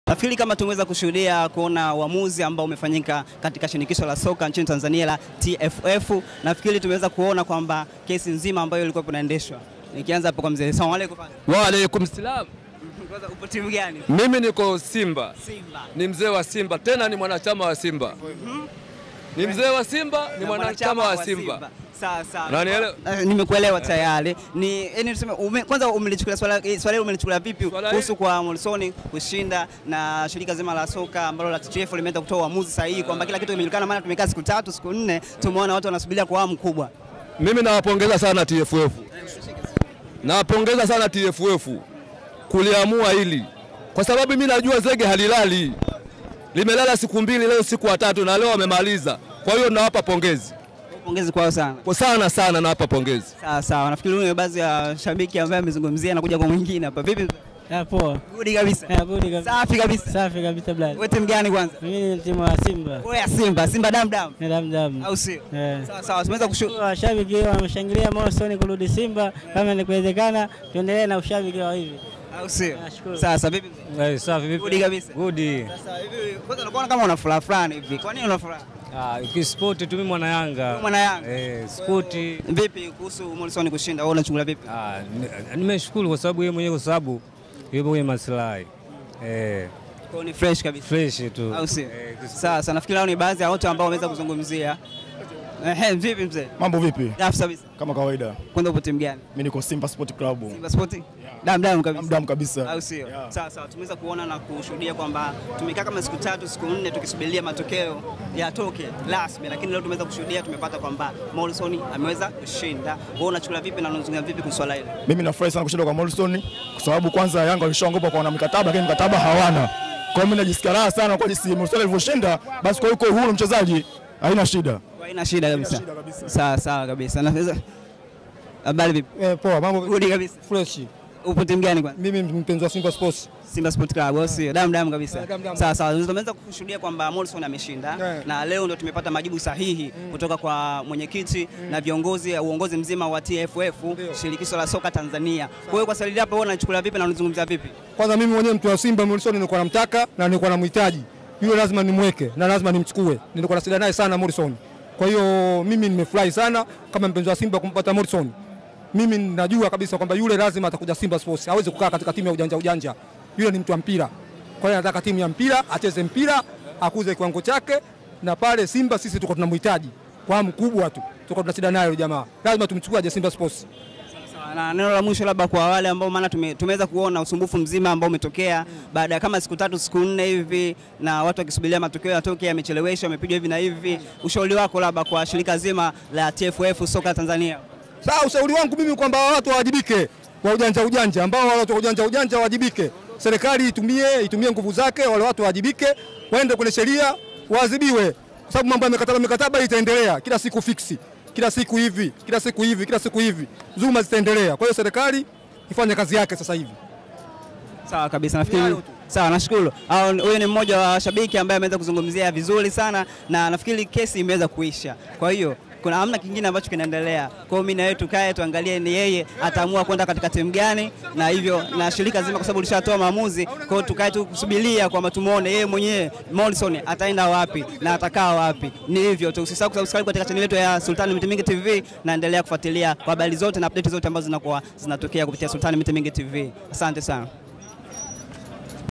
TFF YAMPA USHINDI MORRISON YUPO HURU YANGA WALIA KUPOTEZA MDA TFF SIKU 3 MFULULIZO SIMBA FURAHA TELE
KAZI IMEISHA KILICHOBAKI TUANGALIE VITU VINGINE DAKIKA 90 ZIMEISHA MUAMUZI HII NI KAZI NDOGO ILA UNAJUA HAWA NIWATANI TUU TUENDELEE KUTANIANA AU MNASEMAJE WANANCHI